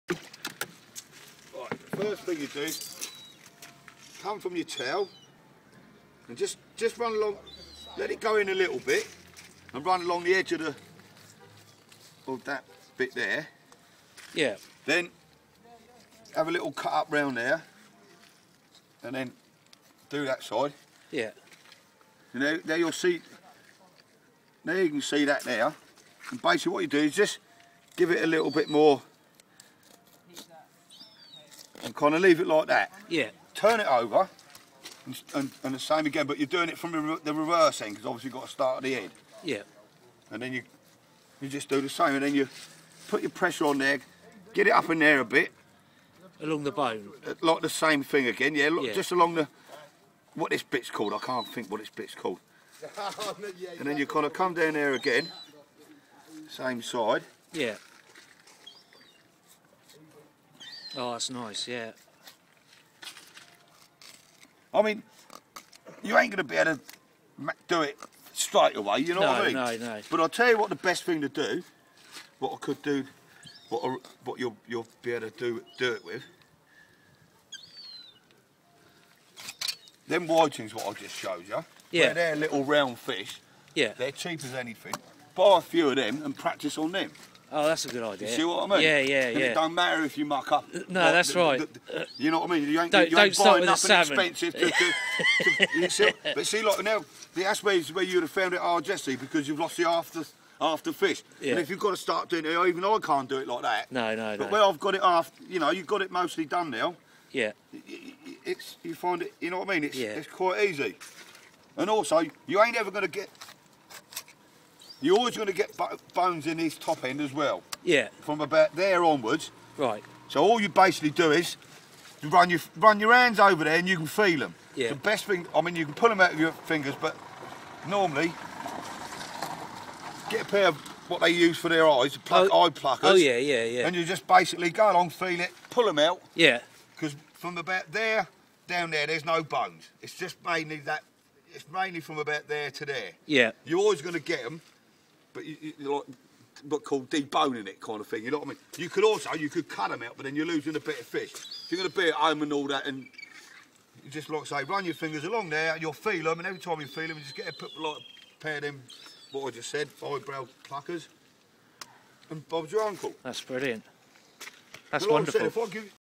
How To Fillet A Round Fish
The trouble with learning how to fillet fish is there are so many different methods. But if you really want to know how to do it, don't ask a celebrity chef who has long since fled the kitchen, ask a fisherman - they do it every day. Here, Billy The Fish shows apprentice chef Alex Watts how to fillet a cod the right way...